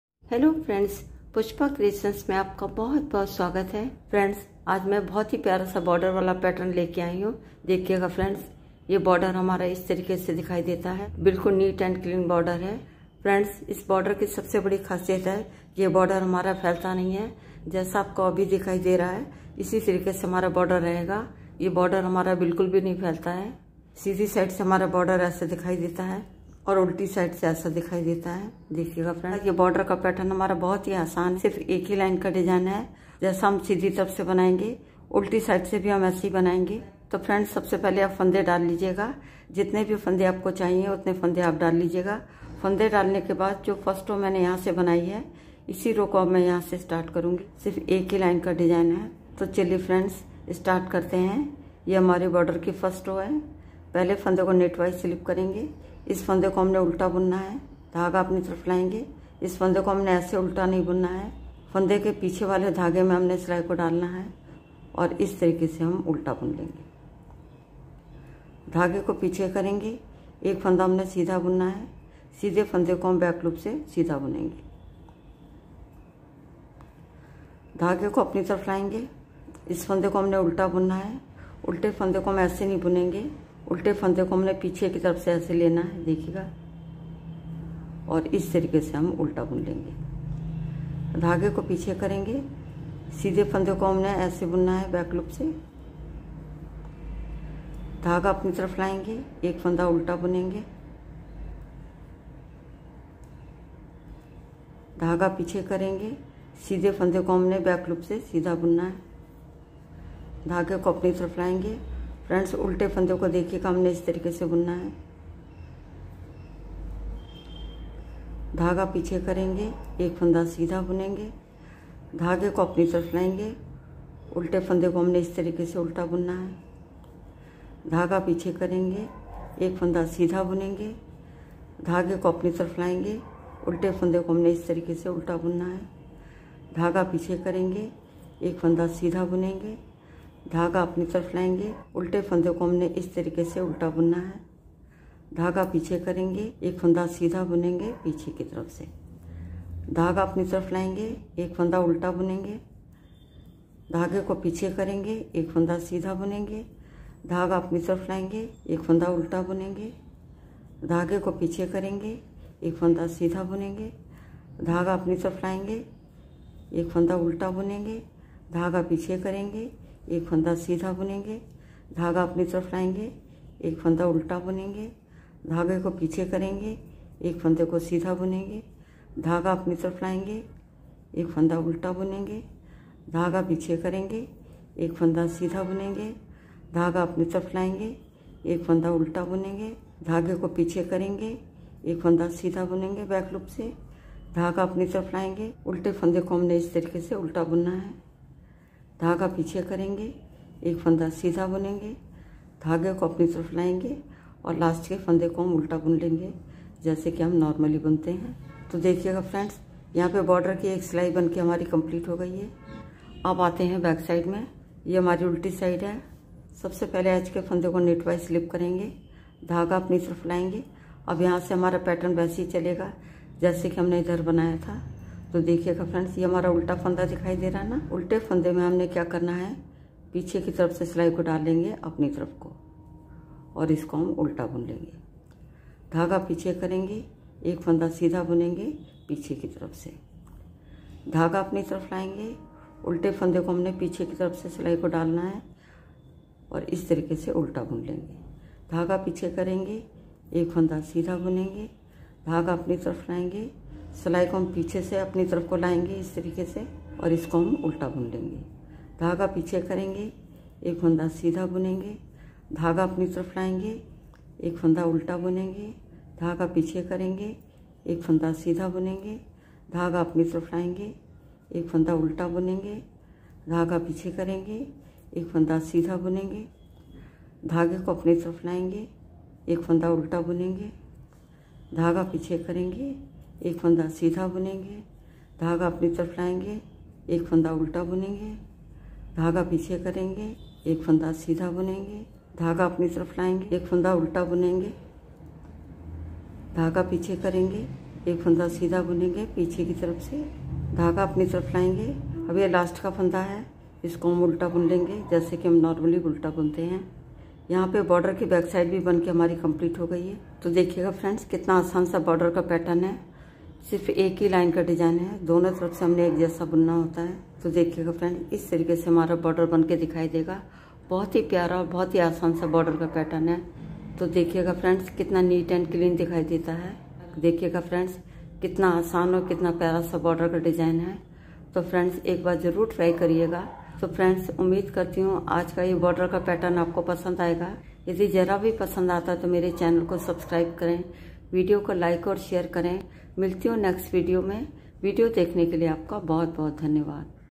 कभी न फैलने वाला बॉर्डर(Clean Border)Only One Row knitting Border Design.Cardigan/Sweater/Jackets.
Clean Border Bunai for Cardigan/Sweater/Jackets.